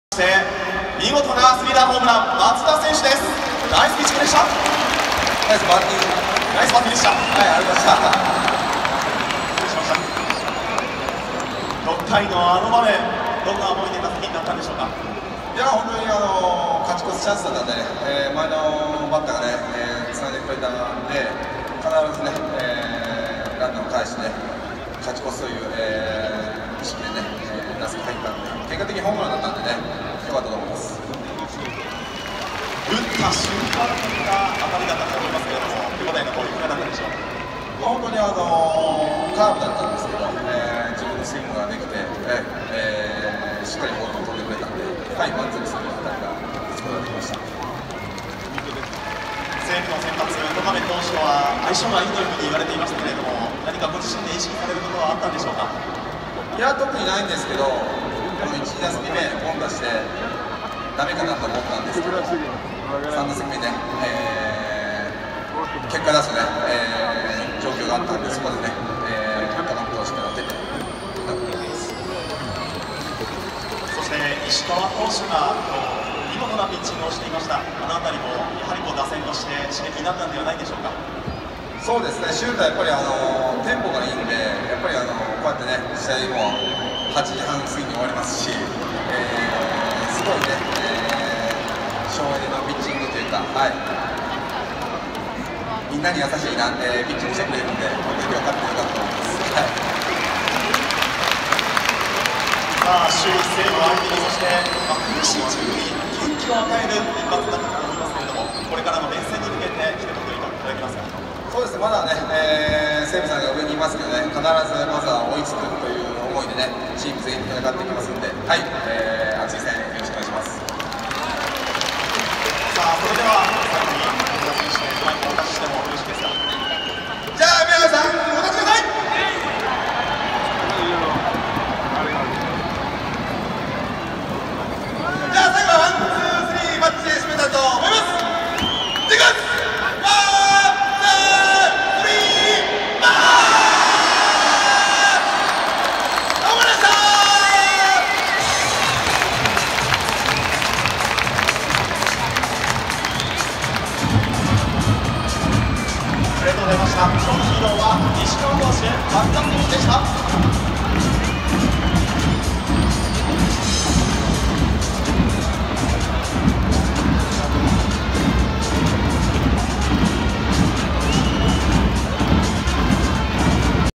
【ヒーローインタビュー・松田】試合が早く終わったことに喜びを隠せない松田（笑）
2018/04/26ホークスVS西武ライオンズ